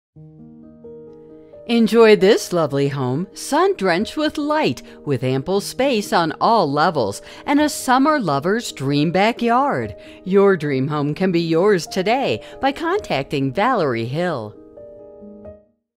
127 Linda Road Braintree, MA | ColdwellBankerHomes.com
127 Linda Road, Braintree, MA

Sun drenched with light, ample space on all levels, a summer lovers dream back yard - check, check, and check! Located in the desirable Braintree Highlands, this home features many amenities including C/A, fully fenced in yard, and inground swimming pool to name a few. Open floor plan w/cathedral ceilin